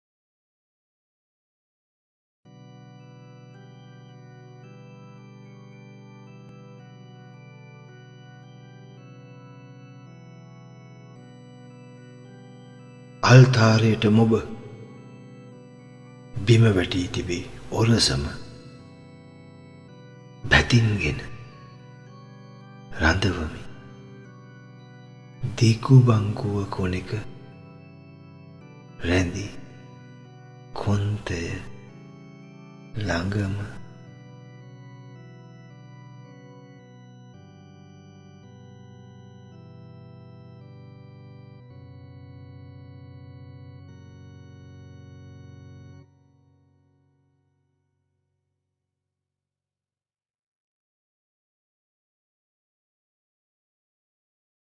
'නිමිති' | 92 | Manjula Wediwardena | මංජුල වෙඩිවර්ධන
'නිමිති' | 92 |

අල්තාරයට මොබ
බිම වැටී තිබි ඔරසම
බැතින් ගෙන රඳවමි
දිගු බංකුව කොණෙක
රැඳි කොන්තය ළඟ ම.

'නිමිති'
විදර්ශන ප්‍රකාශනයකි

'නිමිති'
Written by Manjula Wediwardena, 2020
Read by  Manjula Wediwardena